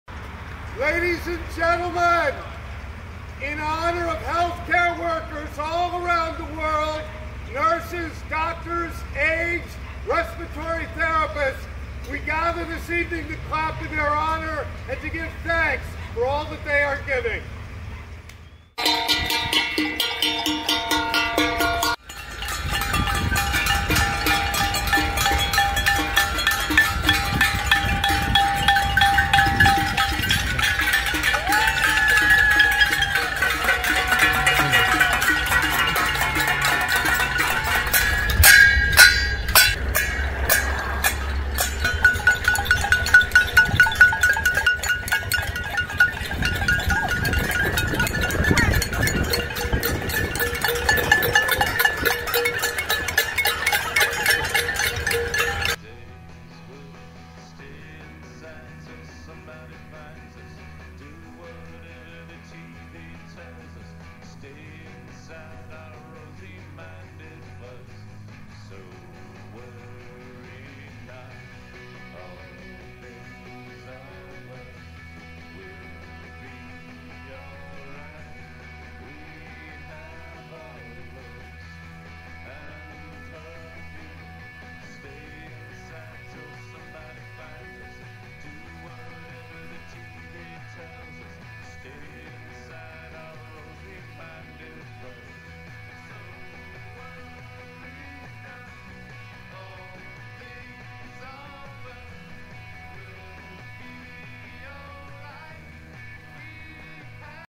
Mercer Street residents in Princeton thank healthcare workers
Residents of Mercer Street in Princeton make some noise to thank healthcare workers.